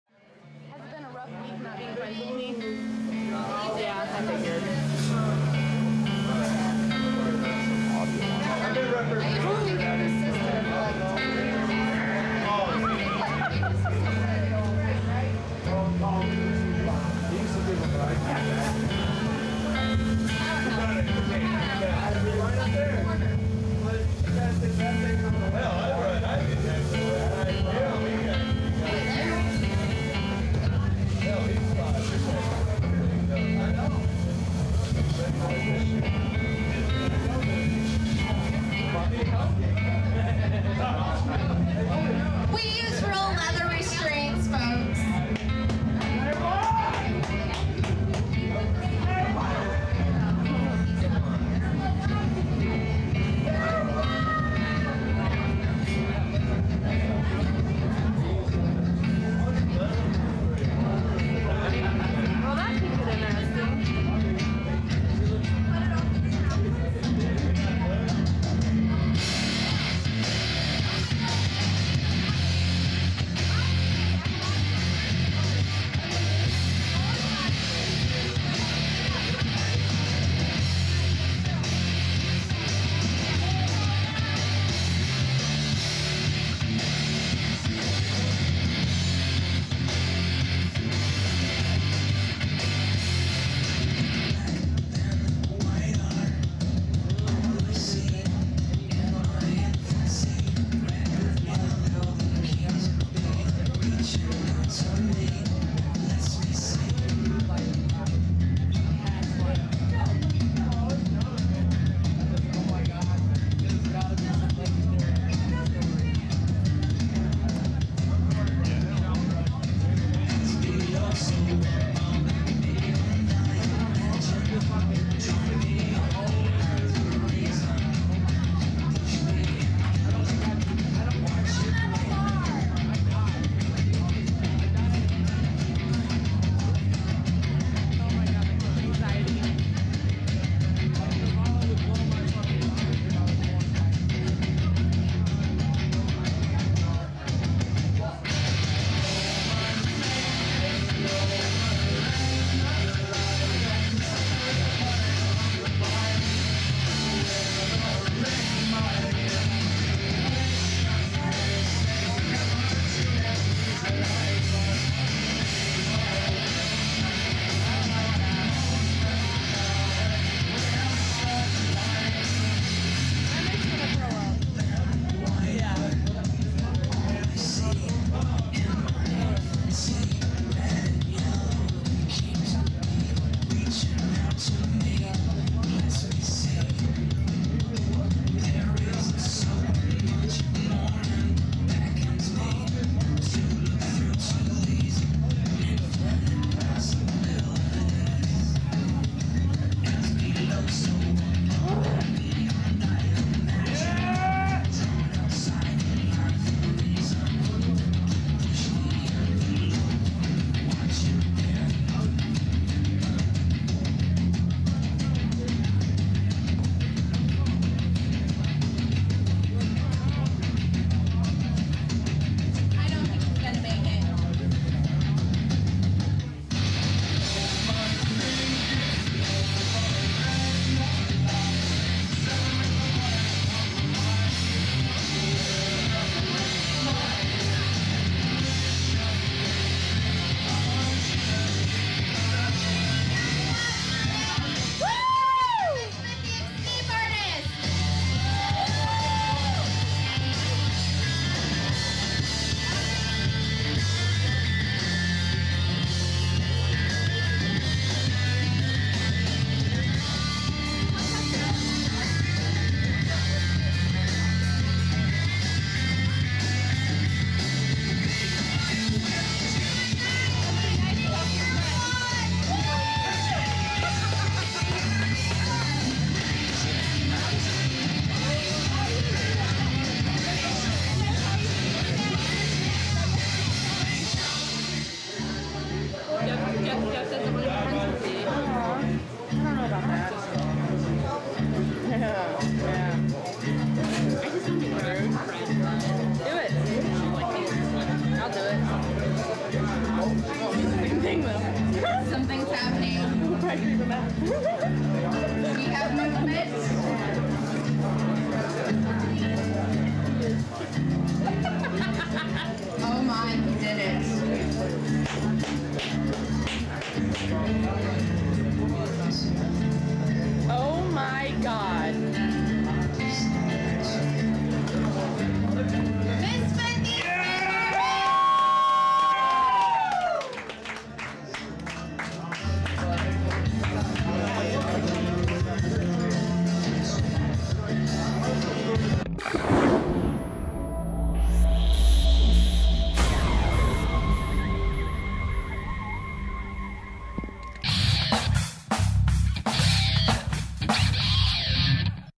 Misfit - the Escape Artist: Mailbag Escape
Misfit performing at Carl's Tavern in New Haven, IN on April 26, 2013.